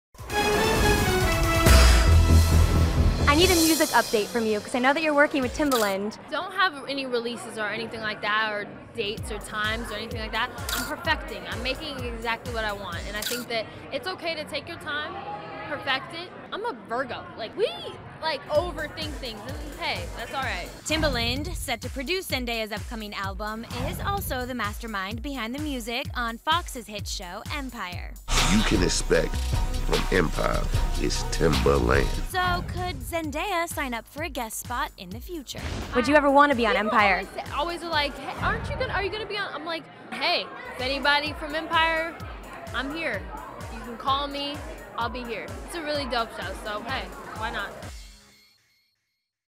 Zendaya On Her New Music With Timbaland, And Will She Guest Star on 'Empire'?
The 19-year-old actress and singer is back in the studio - with Empire's heavy-hitting producer.